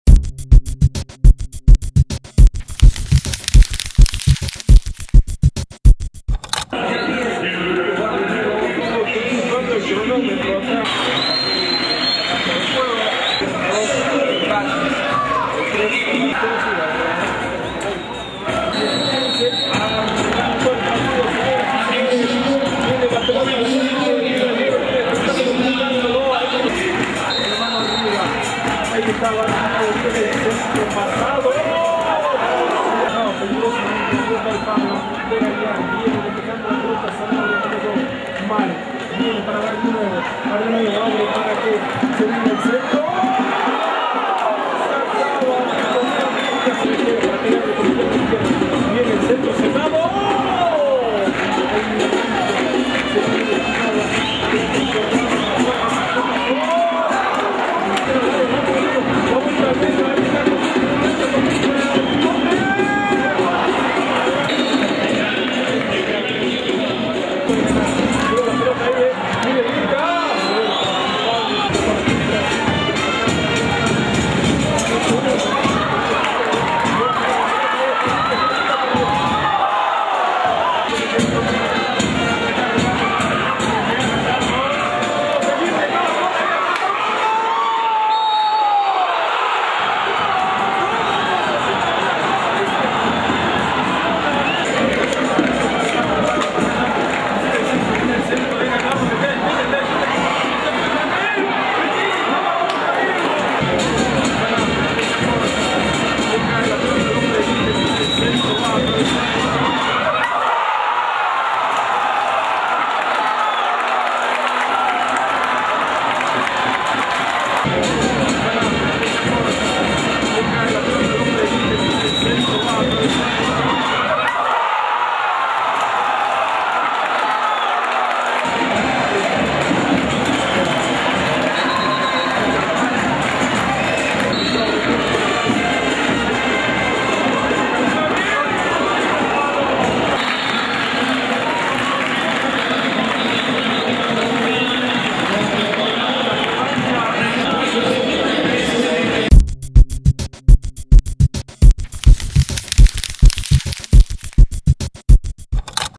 Mitre 1 - 0 Talleres de Perico
Con un gol en el epílogo de Gustavo "Mudo" Barrionuevo el "Ciclón" se quedó con el primer choque de cuartos de final del Federal "B".